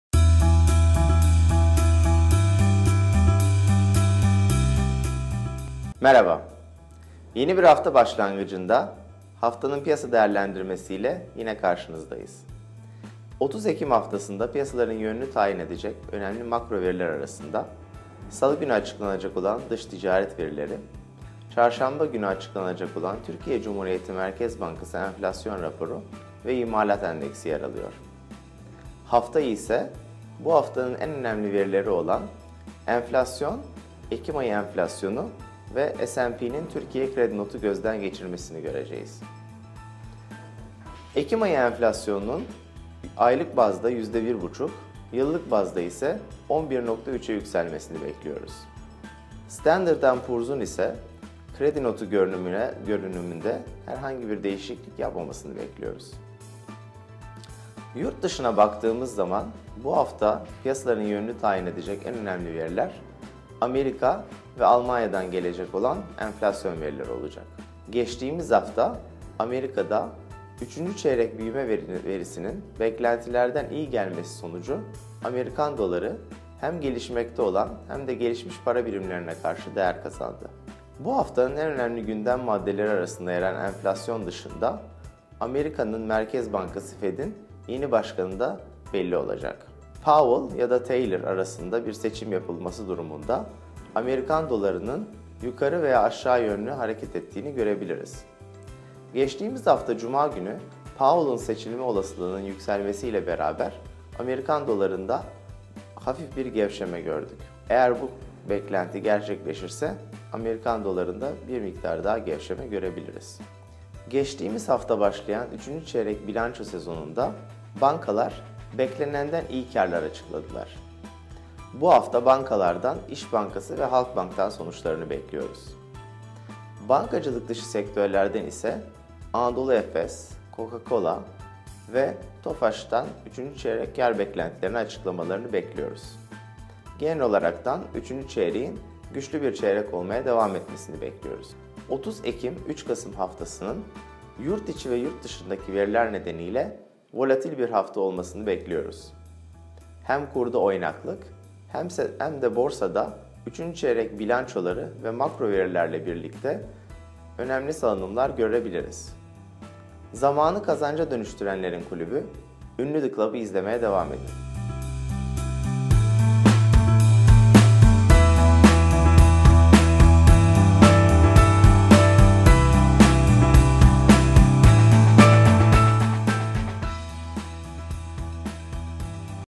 30 Ekim Haftası Piyasa Gündemi
Haftalık piyasa değerlendirmelerinin ele alındığı Haftaya Bakış videolarıyla piyasadaki güncel gelişmeleri takip ediyoruz.

ÜNLÜ & Co Araştırma Bölümü Yönetici Direktörü Dr. Vedat Mizrahi, 30 Ekim haftası gelişmelerini aktarıyor.